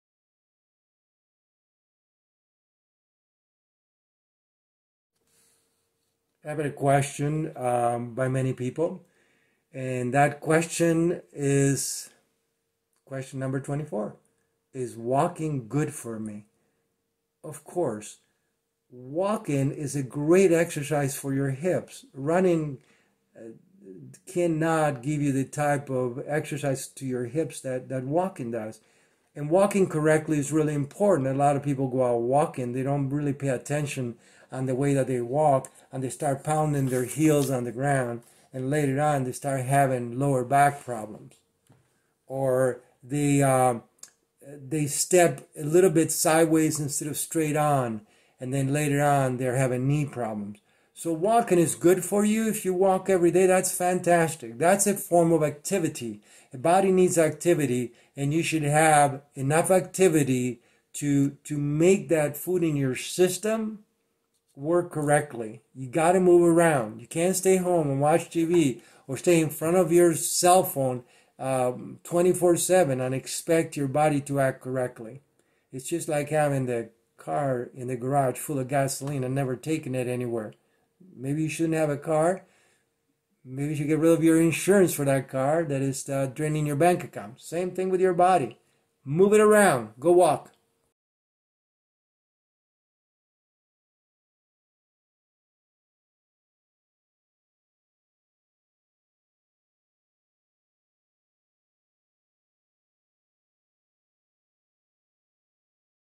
24 - Is Walking Good?
Walking is a great form of exercising.  Walking is good for the hips, for lung expansion and for strengthening the legs.  Caution should be taken when going for a walk that we don’t get so distracted that we step incorrectly, with our feet facing outwards of planting the feet too hard on the ground when we start getting tired.  Both those things can cause knee and back damage.